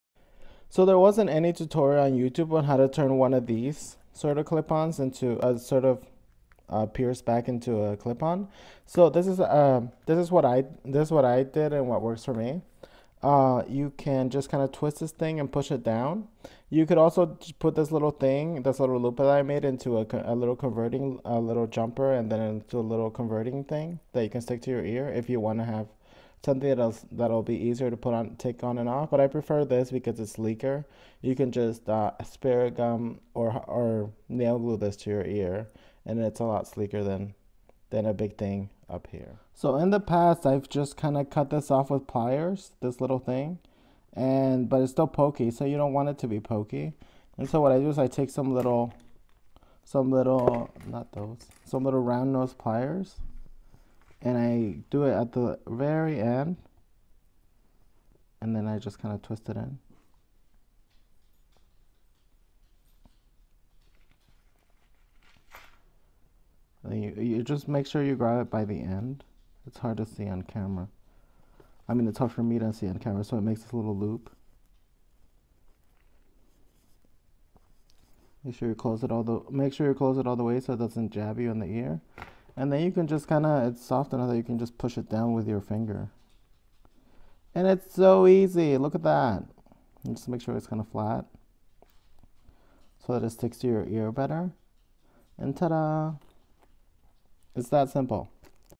Converting needle back earrings drag tutorial
How to convert needle back pierced earrings to glue on/clip on without it looking bulky and not cute.

Didn't show how to add a jump ring and a clip on attachment because there's a million tutorials already on how to do that.